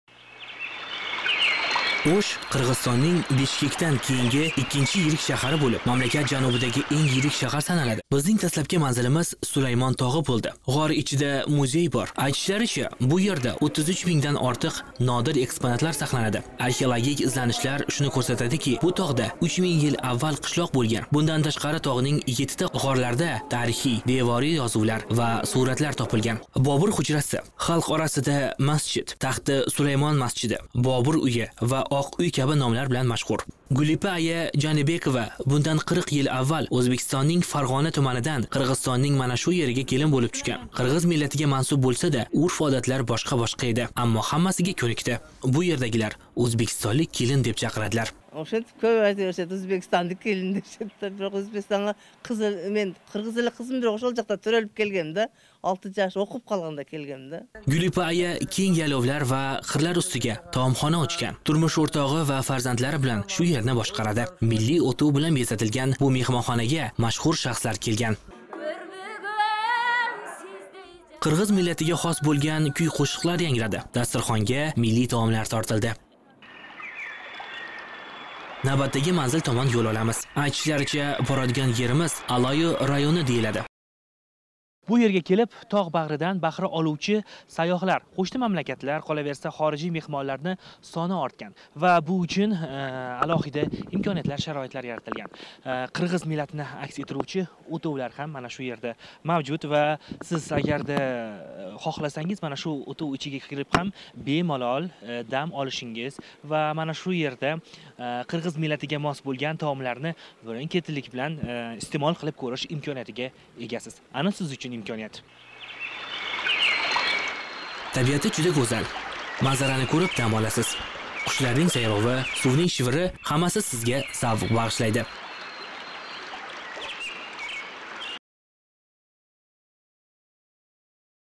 قیرغیزستان نینگ اۉزبېکلر یشه‌یاتگن اۉش شهری
Qirg‘iziston ning o'sh shaharga safar
اۉزبېکستان ژورنالیست احرار ضیا سیزگه بو شهر گوزللیکلرینی کۉرسه تیب بېره دی.